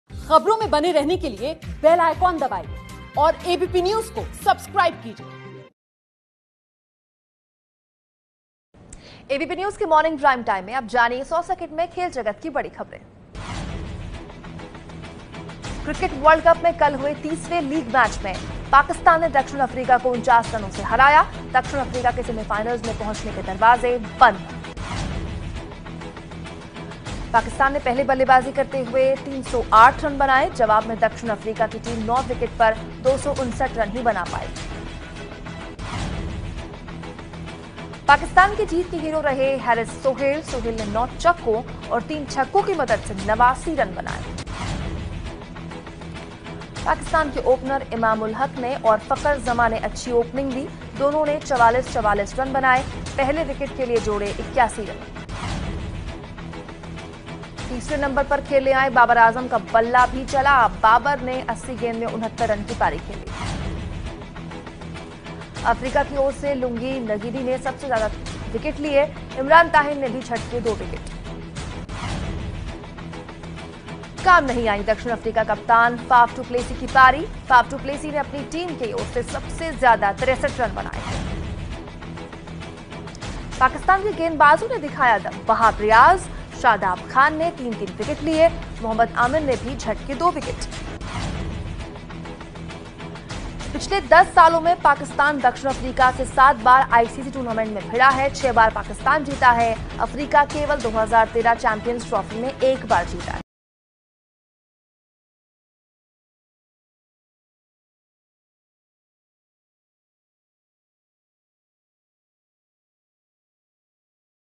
वर्ल्ड कप में साउथ अफ्रीका के लिए सेमीफाइनल के दरवाजे हुए बंद, 100 सेकंड में देखिए खेल की बड़ी खबरें
-क्रिकेट विश्वकप में कल हुए 30वें लीग मैच में पाकिस्तान ने दक्षिण अफ्रीका को उनचास रनों से हराया, दक्षिण अफ्रीका के सेमीफाइनल में पहुंचने के दरवाजे बंद.
-पाकिस्तान ने पहले बल्लेबाजी करते हुए तीन सौ आठ रन बनाए. जवाब में दक्षिण अफ्रीका की टीम 9 विकेट पर दो सौ उनसठ रन बना सकी.
-पाकिस्तान की जीत के हीरो रहे हैरिस सोहेल. सोहेल ने 9 चौकों और तीन छक्कों की मदद से नवासी रन बनाए.
-तीसरे नंबर पर खेलने आए बाबर आजम का बल्ला भी चला. बाबर ने 80 गेंद में उनहत्तर रन की पारी खेली.
100 सेकंड में देखिए खेल की खबरें